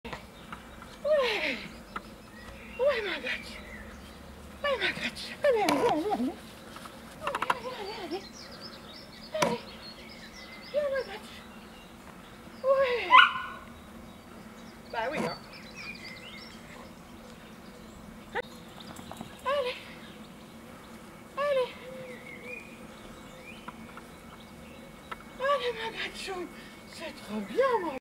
Strato marche en laisse avec tata
Marche en laisse pour Stratocaster (3 mois) avec tata Gretsch
Qui promène qui ?